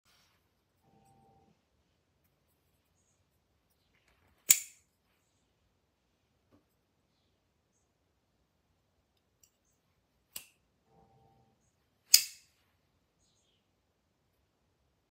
Petrified Fish Titanium Cash Bar!
PETRIFIED FISH CASH FRONT FLIPPER MONEY CLIP POCKET KNIFE GRAY TITANIUM HANDLE 14C28N PLAIN EDGE SATIN FINISH E09-GAS

$72.99 before discount

Blade Length: 1.81"

Blade Width: 0.73"

Blade Thickness: 0.08"

Blade Material: 14C28N

Plain Edge

Satin Finish

59-61HRC

Flat Grind

Gray Titanium Handle

Frame Lock

Weight: 1.69oz

Designed By: Tiguass

TWOSUN TS241 BALL LOCK KNIFE TITANIUM + CARBON FIBER HANDLE PLAIN S90V BLADE

$89.99 before discount

DESCRIPTION:
Overall Length: 6.46", 164 mm

Handle Length: 3.82", 97 mm

Blade Length: 2.6", 66 mm

Ball Lock

Blade Steel: S90V

Handle Material: Carbon Fiber & Titanium

Weight: 1.94oz, 55 g

KVT Ceramics Ball Bearings

Manufactured in China